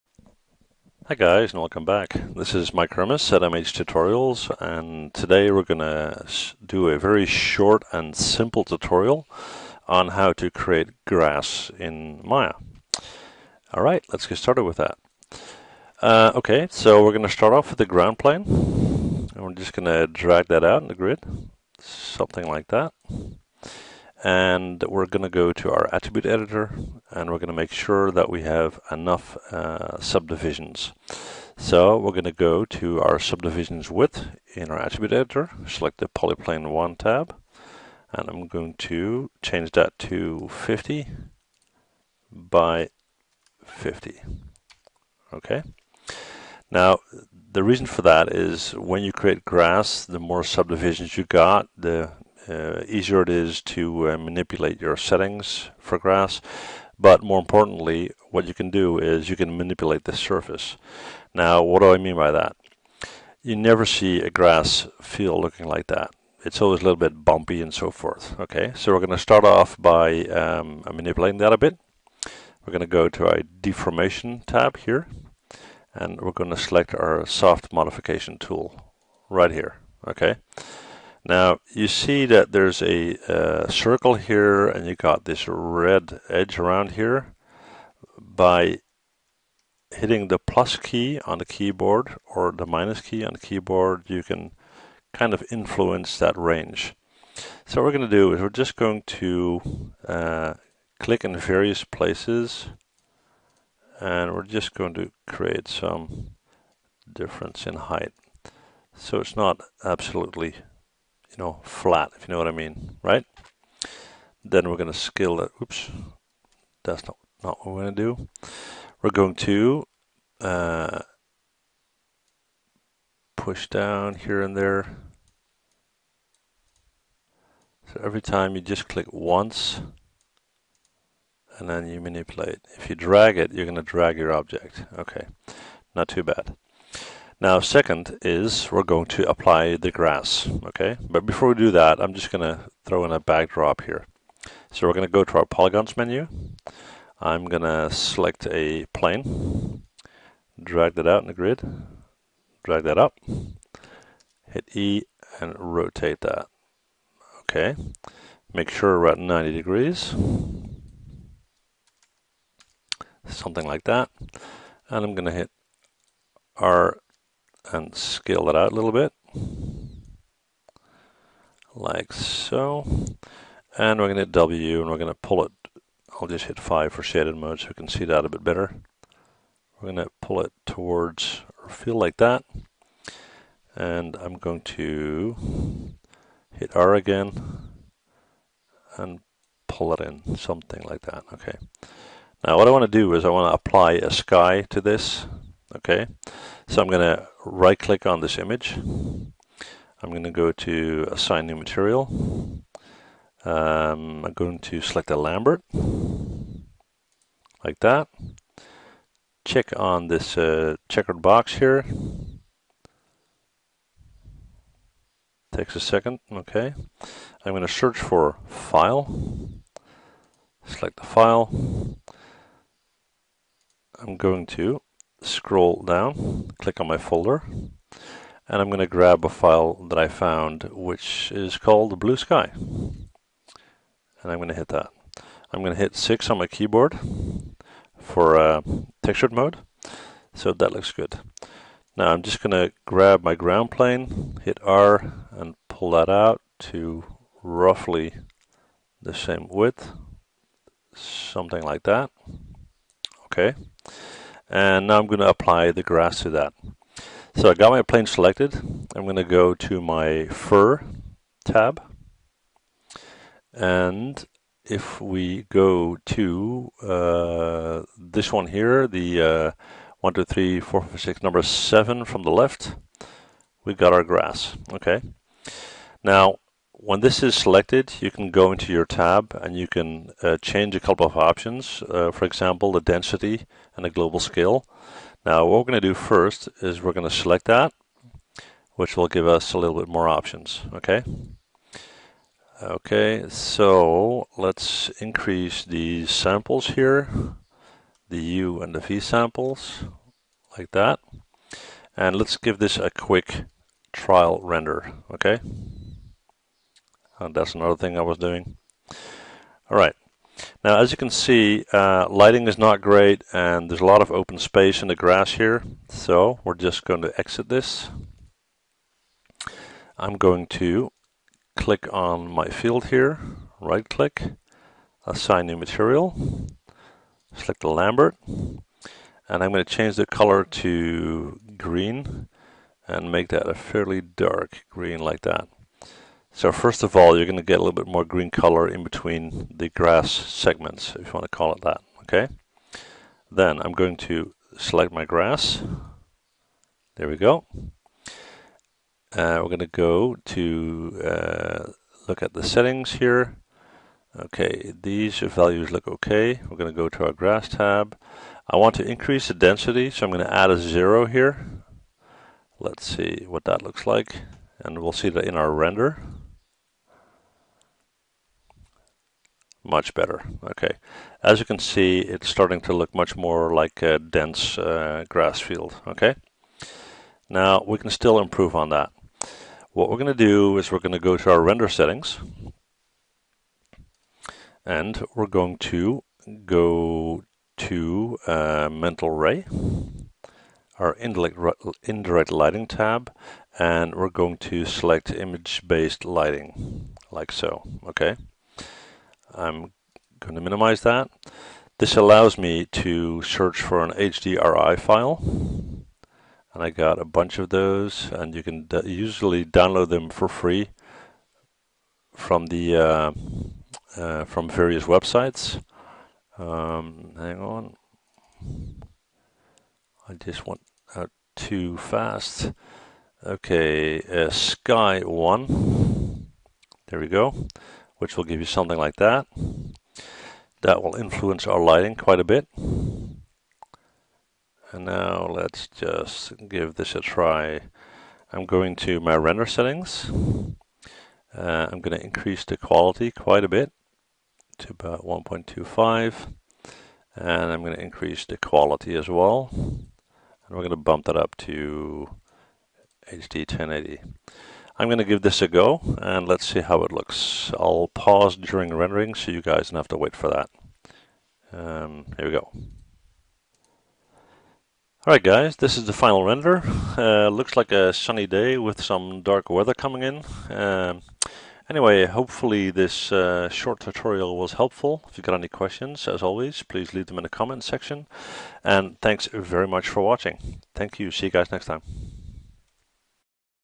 Maya 2014 tutorial : How to create and use grass in your scene
In this short tutorial I will explain how to prepare your ground surface, apply grass to it, and set it up in a scene.